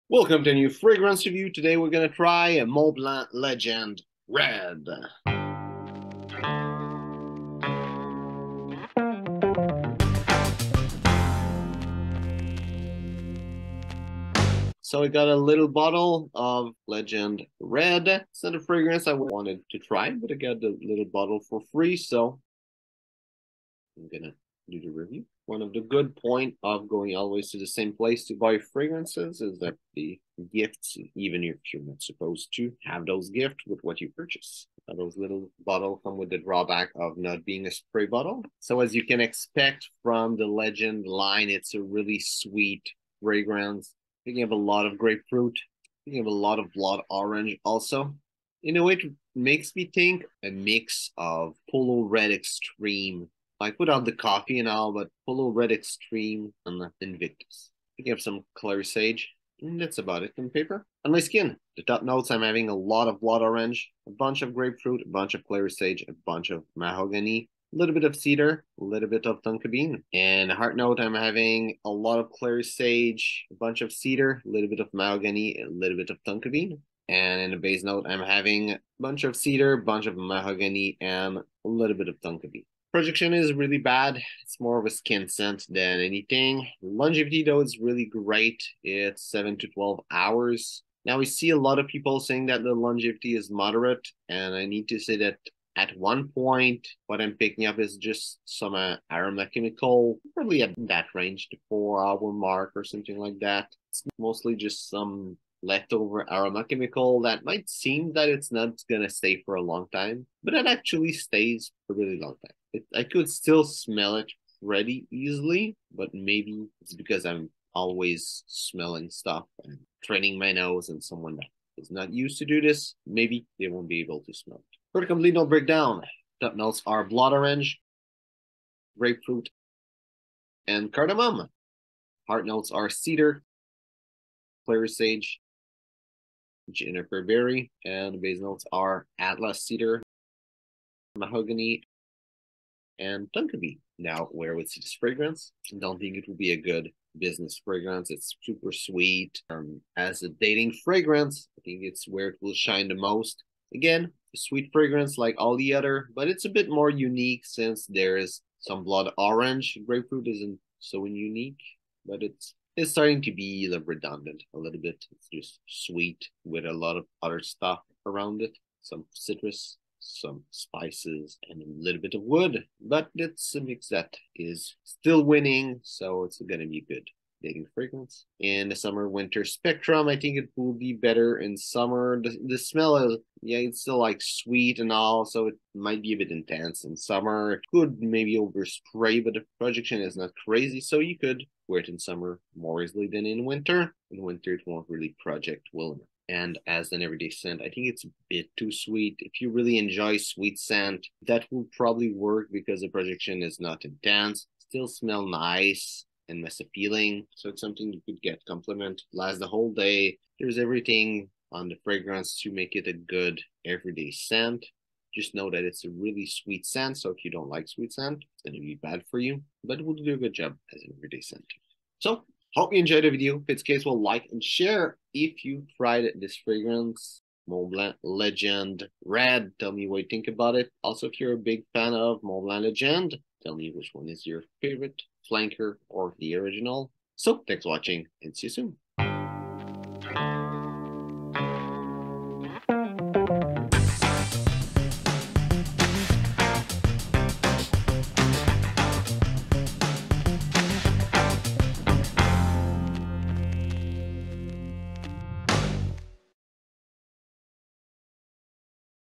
POLO RED AND INVICTUS' BABY - LEGEND RED BY MONTBLANC
What is your favorite MontBlanc Legend flanker?

To get a great deal on many fragrances and a $5 off coupon on your first purchase